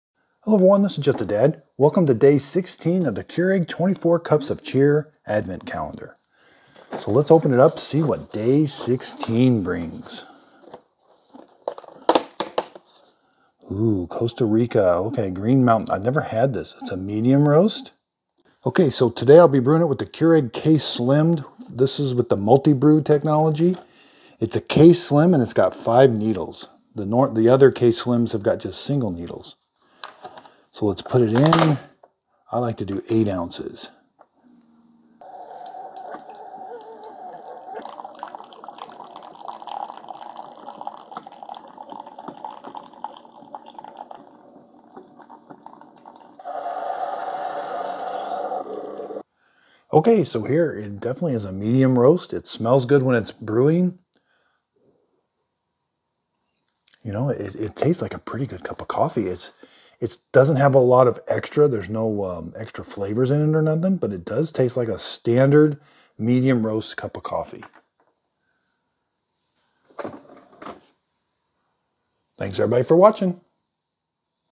DAY 16 Keurig Advent Calendar 24 Cups of Cheer Coffee K Cups 25 Days of Christmas K-Slim
Green Mountain Coffee Each Day I will post a video of what is behind the secret compartment.  Keurig Advent Calendar Variety Pack, Single Serve K-Cup Pods, 24 Count Today it was Green Mountain Coffee Roasters Costa Rica Paraiso, Single-Serve Keurig K-Cup Pods, Medium Roast Coffee Pods,